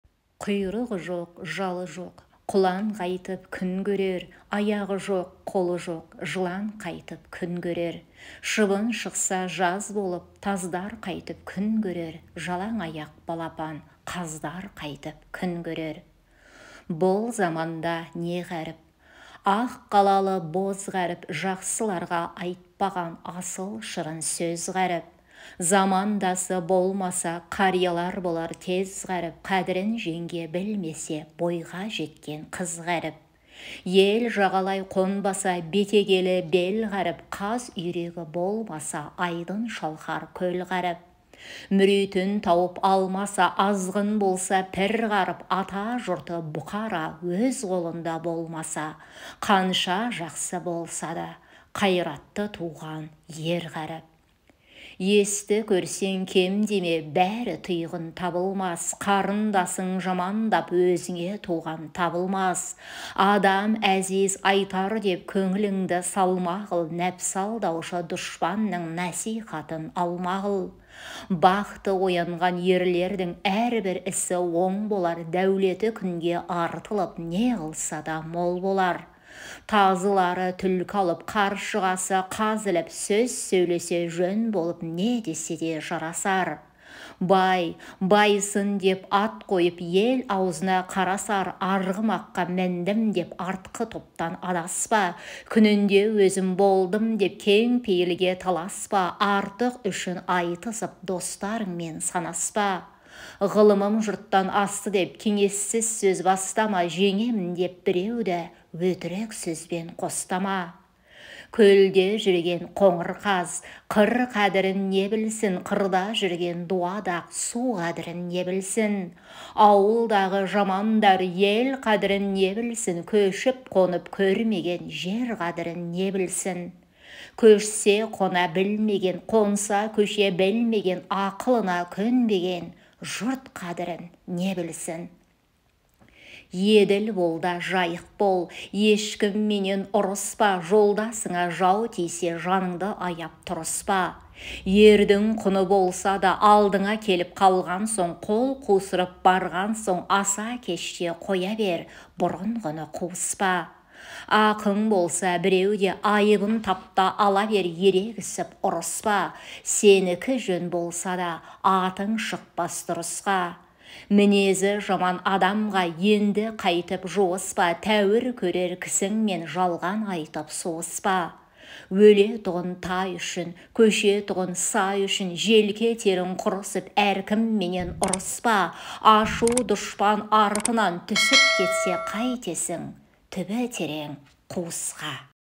Асан қайғы жырларынан үзінді
Асан қайғы (Хасан) Сәбитұлы (14 ғасырдың ақыры – 15 ғасырдың басы) – ақын, жырау, би, философ. Әз Жәнібек ханның ақылшысы болған.
Қызылорда облысы Шиелі ауданы «Жеті әулие» қорымындағы Асан ата кесенесі - Асан Қайғы мазары делінеді.